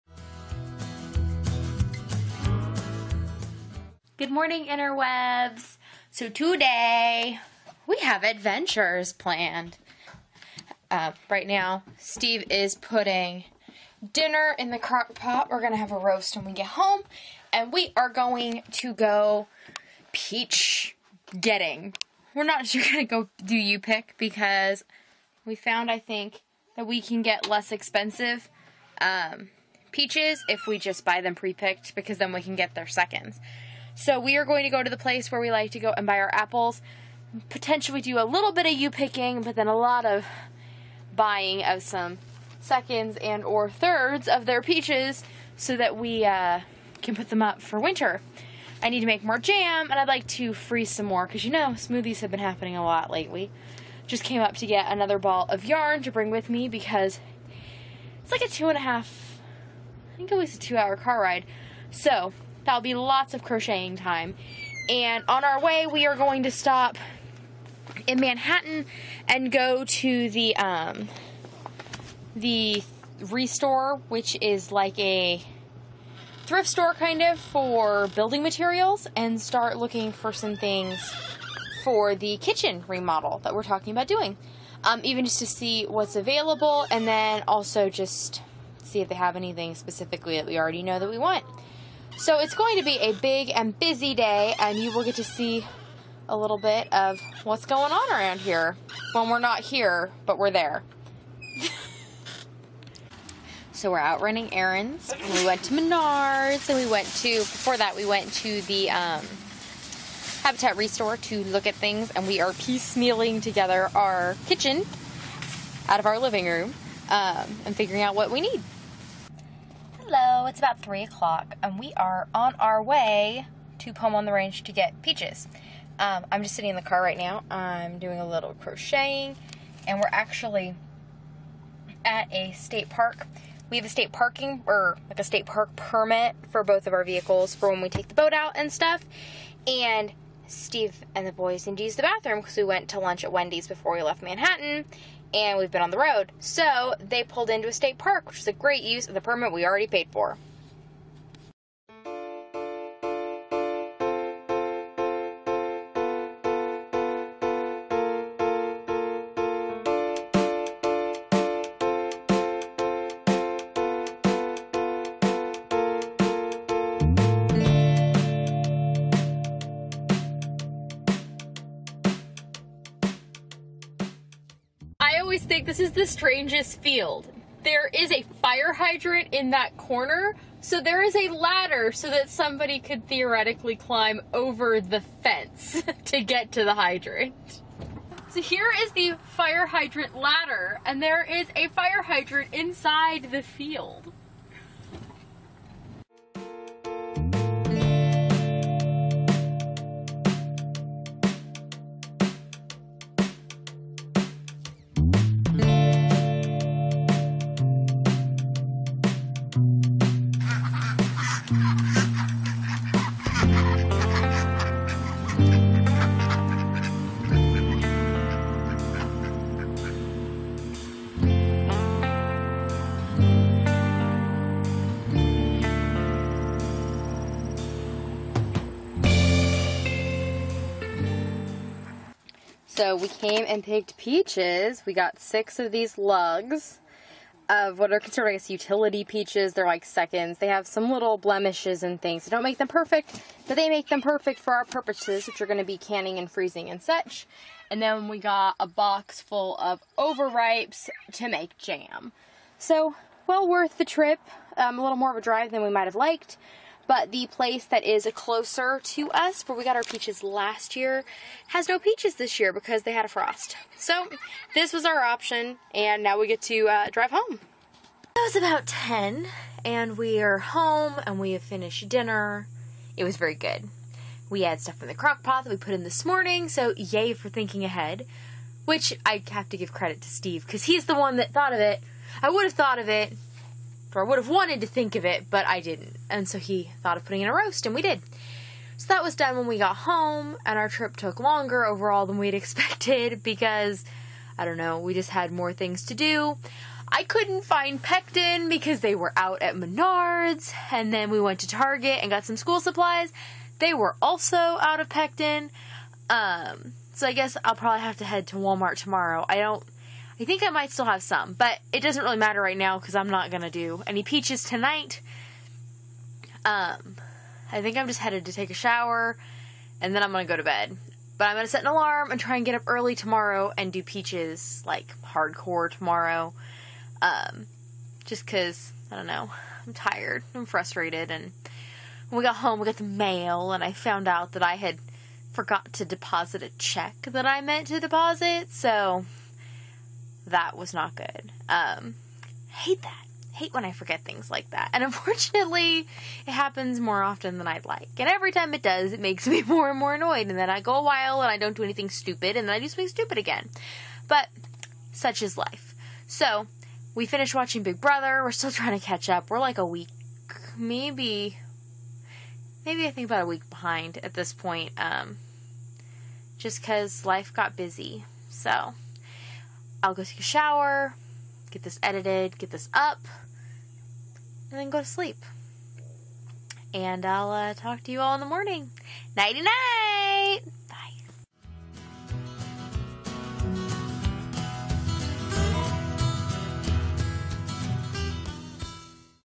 Daily Vlog 8.5.16: Peachy Keen [Vlogust Day 5]
Looking at my plans for August we have a busy month ahead. There's a number of cleaning, decorating, and home organization tasks I need to tackle. I think daily vlogging will help keep me accountable.